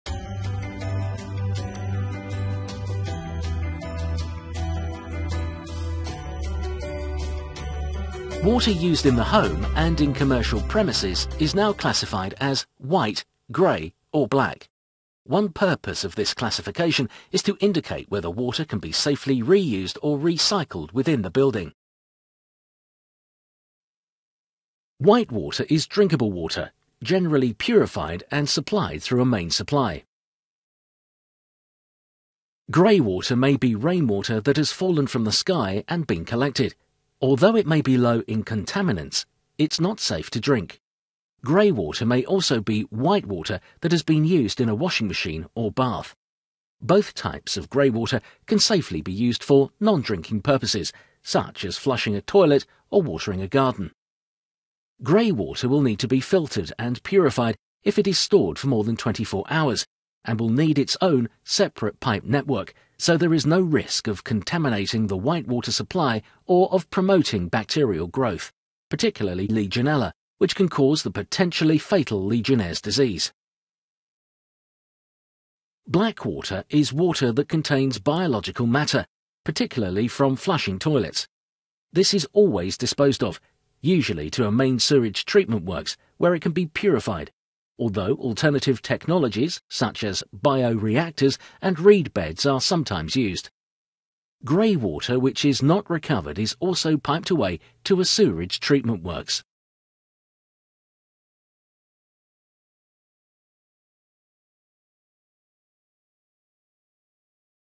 White Grey Black Water
Explaining the different types of water relevant to water saving and water recycling. Produced for the e-SKILL project.